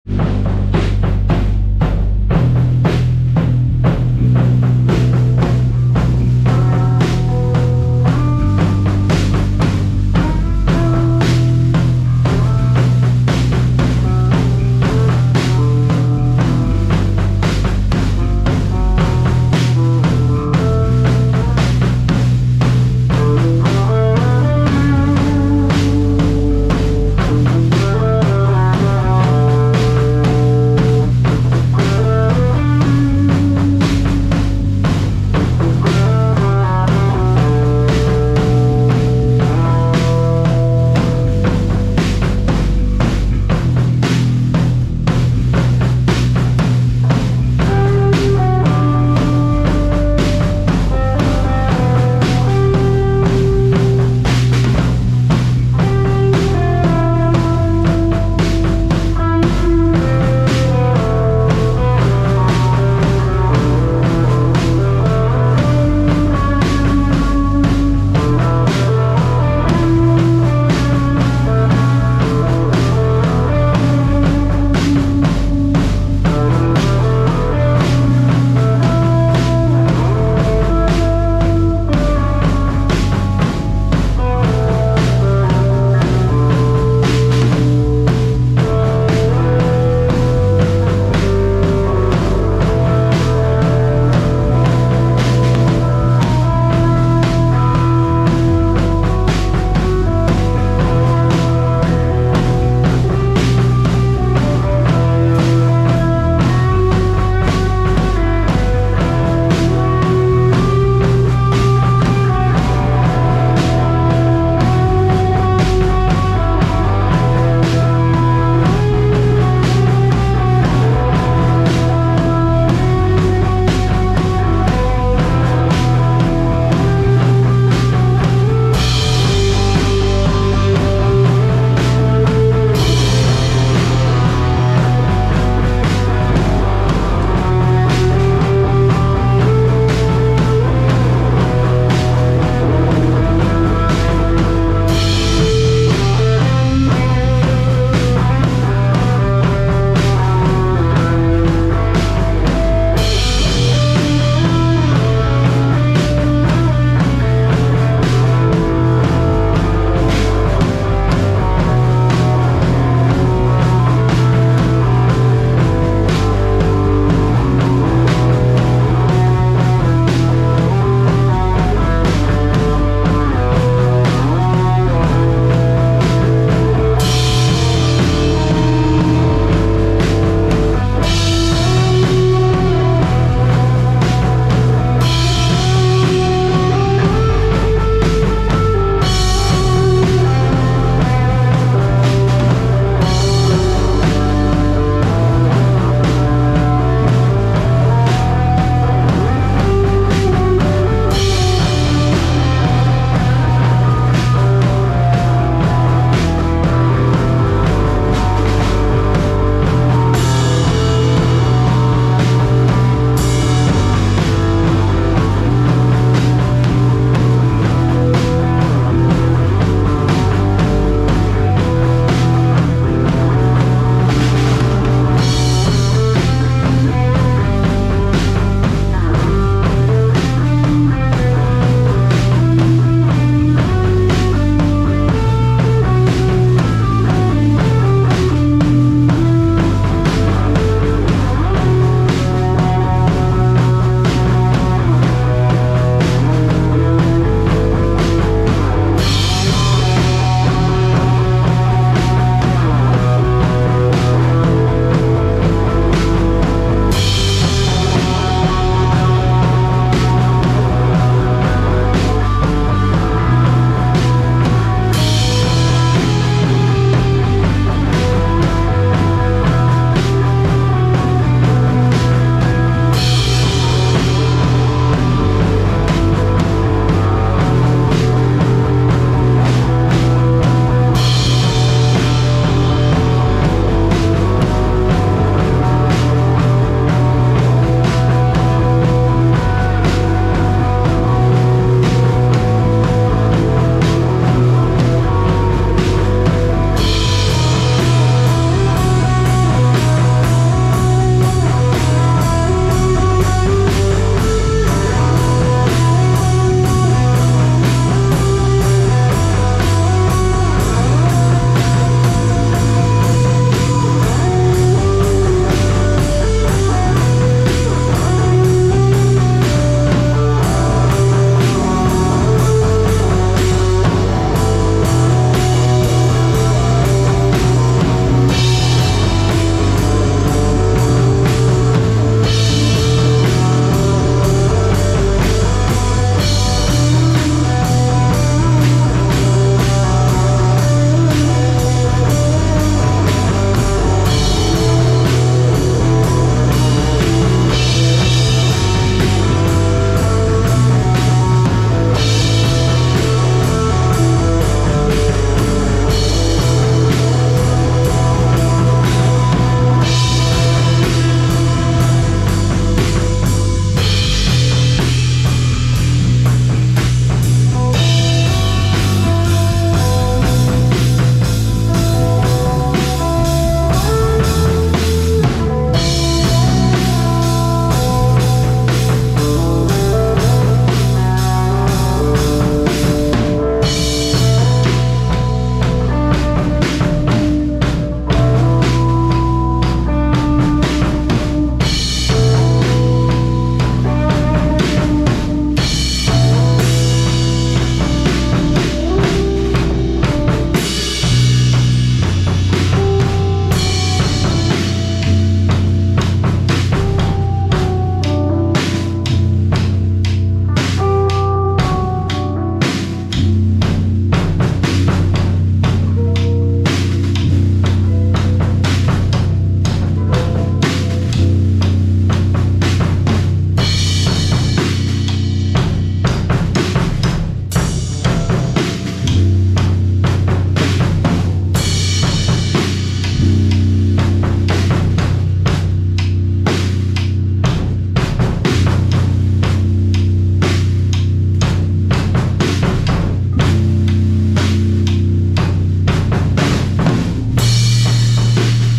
Dirt Melt - Jam 1 (Practice 10.30.23)
Dirt Melt is:
Zack A. - guitar, vocals
Matt H. - bass